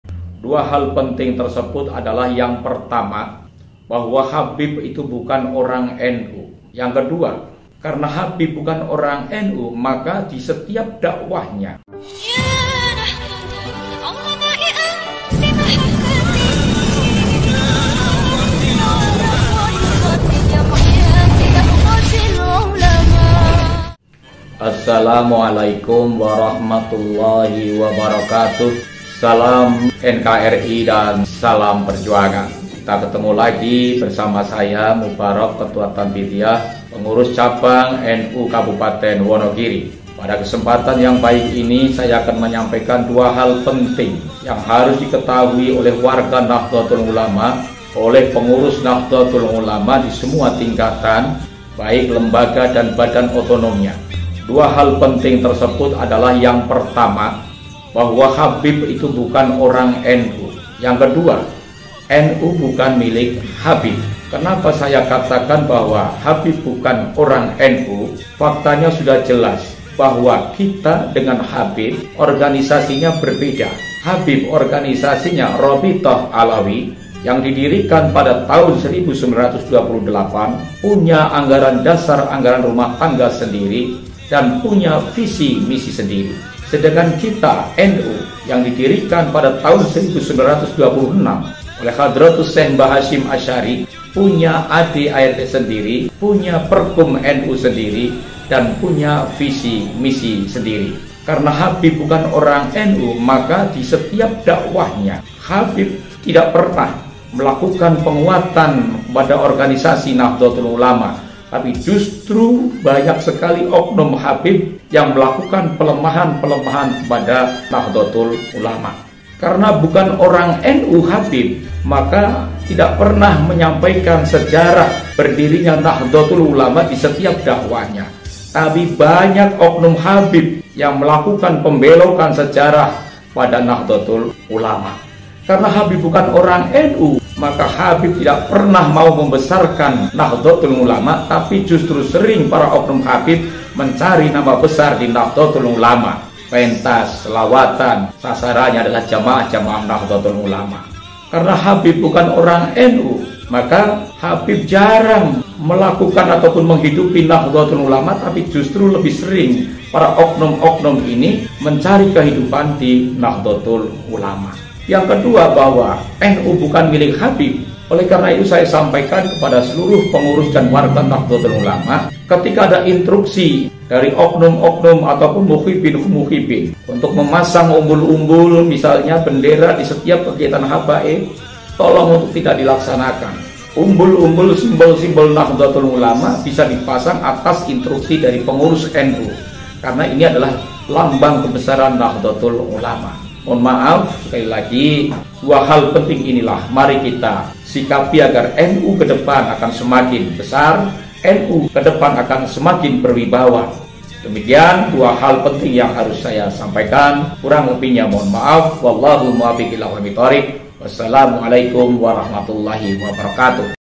Kenapa PCNU Wonogiri Makin Tegas Kepada Habaib⁉️ Simak Ini...‼️
Dua pesan penting Ketua Tanfidziyah PCNU Wonogiri, KH. Mubarok, kepada warga NU dan semua Pengurus NU.